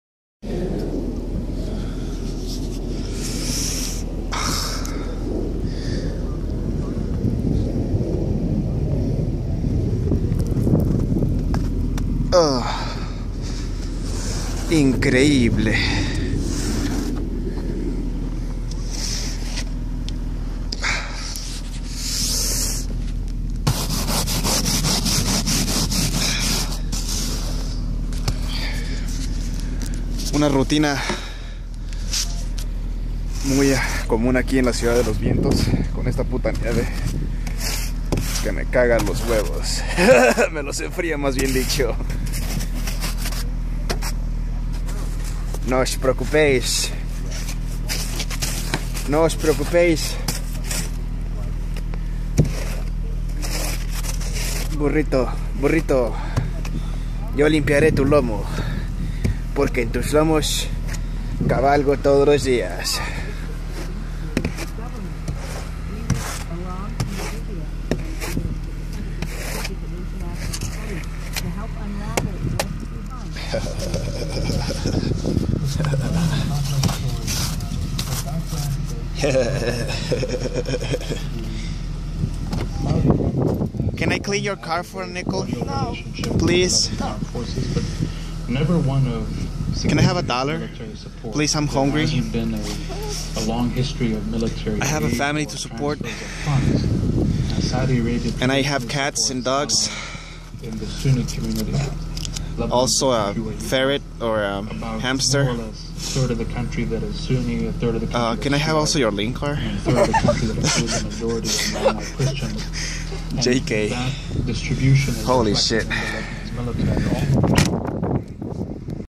Se lo limpio y se lo rasco por un peso!
Chicago's first snow of 2014

Trying to make a living out of this weather conditions.